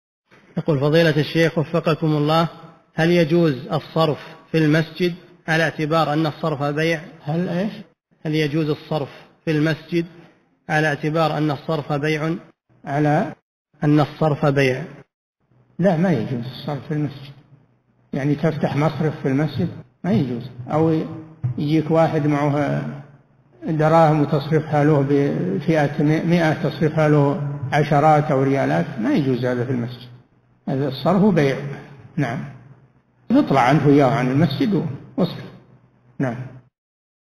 [1220 -2802] هل يجوز الصرف في المسجد على اعتبار أن الصرف بيع؟ - الشيخ صالح الفوزان
السؤال: هل يجوز الصرف في المسجد على اعتبار أن الصرف بيع؟
الجَوَابُ: لا يجوز الصرف في المسجد، يعني تفتح مصرف في المسجد، أو يأتيك واحد معه دراهم 100 تصرفها له فئة عشرات أو ريالات، لا يجوز هذا في المسجد. الصرف بيع، يطلع عنده تجاره خارج المسجد واصرف.

للاشتراك في قناة الشيخ صالح الفوزان
سؤال وجواب من شرح أخصر المختصرات ( 2802 ) مقطع

للاستفسار والإقتراحات والدعم
رئيس الفريق
لمشروع كبار العلماء